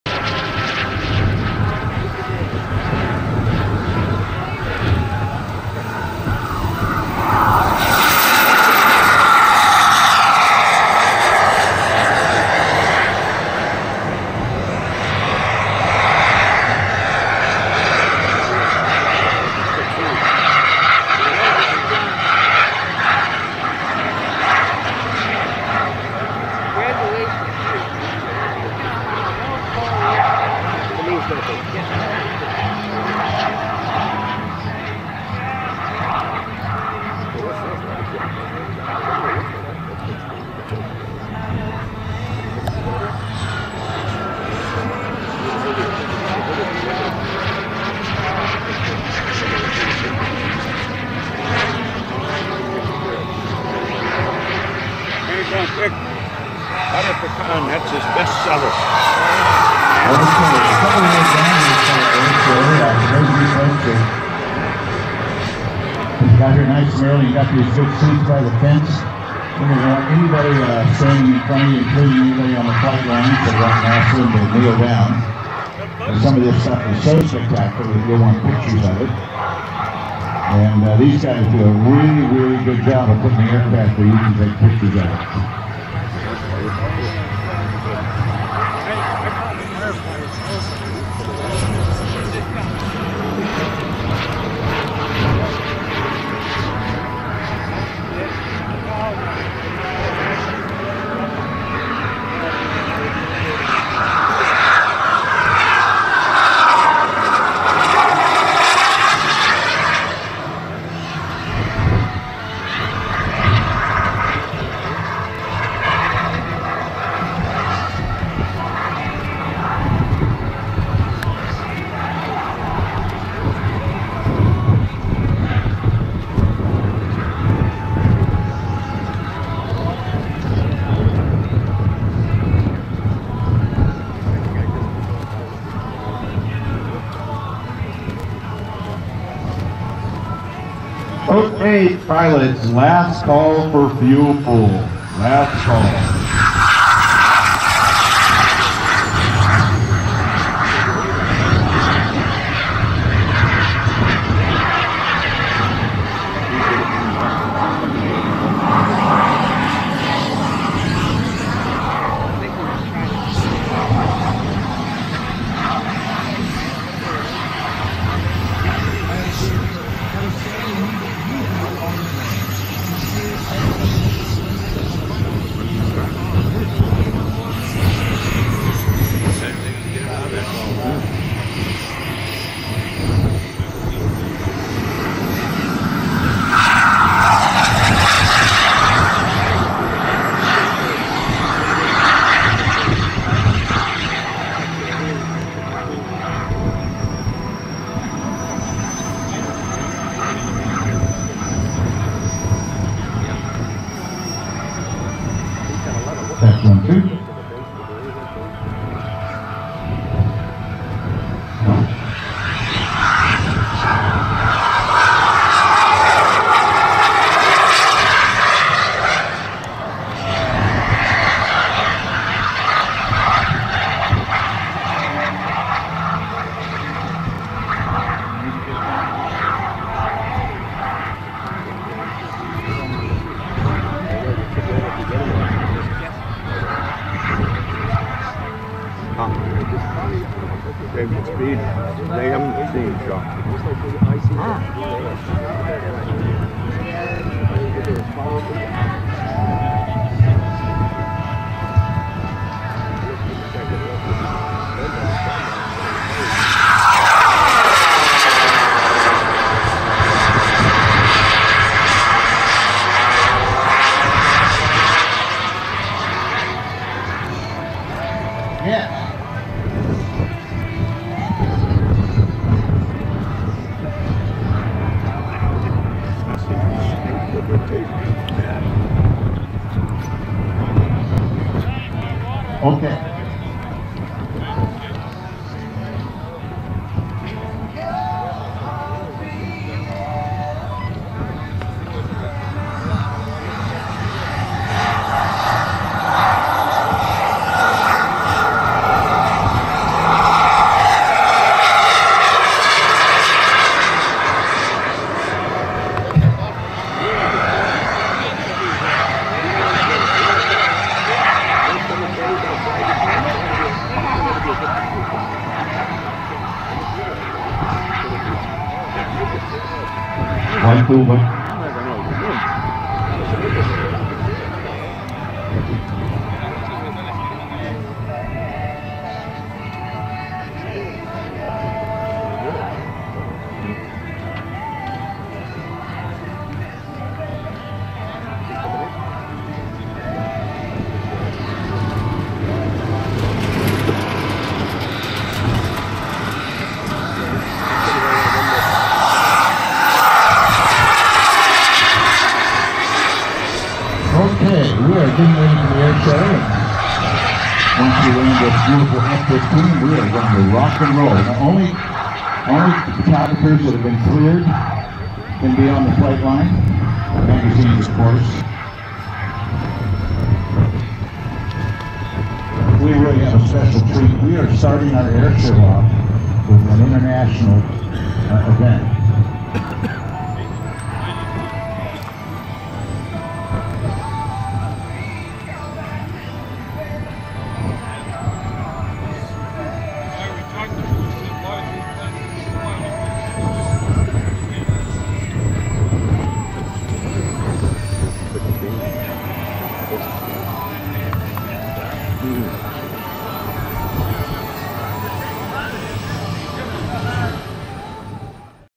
Kris Gunther Feibau F-15
Kris Gunther flying his twin Kingtech powered Feibau F-15
With the music coming through the PA system I'll let YouTube plug in whatever ads they want.